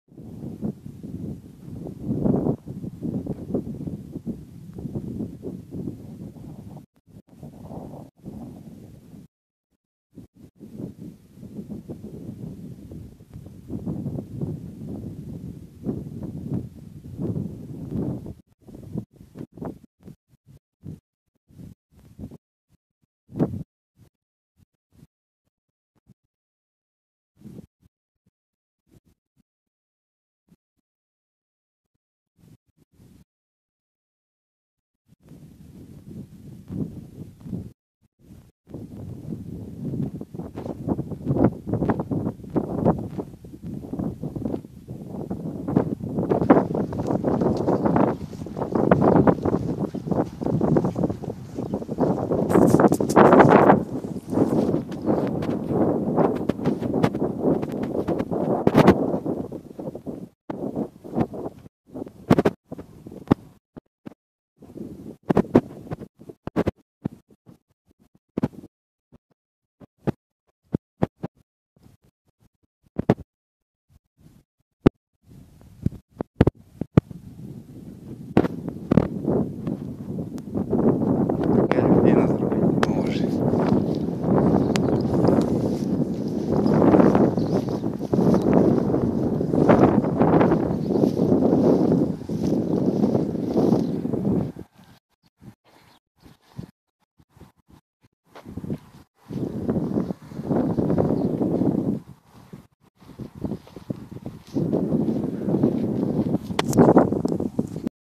земля смиківці
Земельні ділянки, де трапився інцидент, знаходяться за кількасот метрів від новобудов. Відеокамера на одному з будинків зафіксувала машину, яка в ніч із 9-го на 10-те травня близько 4-ої години ранку зробила цей ганебний, варварський вчинок.